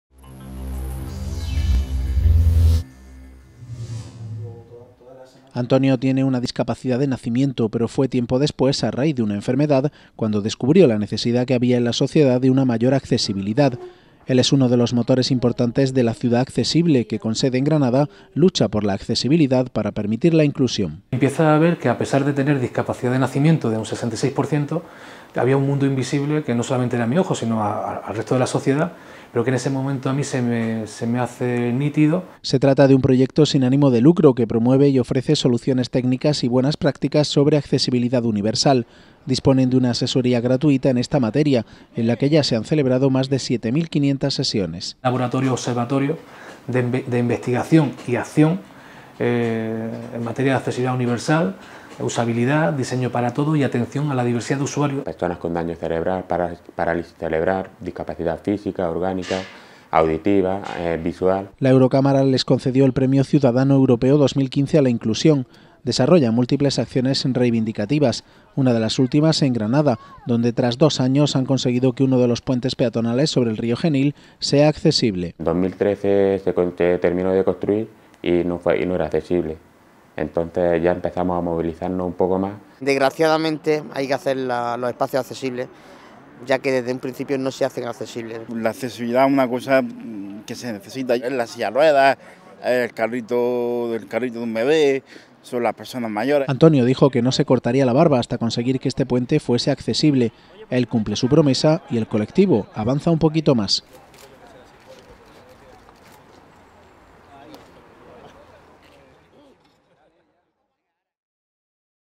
HDL La Ciudad Accesible: un puente a la inclusión social con reivindicación y asesoramiento
La Ciudad Accesible, con sede en Granada, es un proyecto sin ánimo de lucro que trabaja en pro de la accesibilidad universal. Ganadores del Premio Ciudadano Europeo 2015 a la inclusión que otorga la Eurocámara, son protagonistas de numerosas acciones reivindicativas y han realizado más de 7.500 sesiones de asesoramiento de manera gratuita.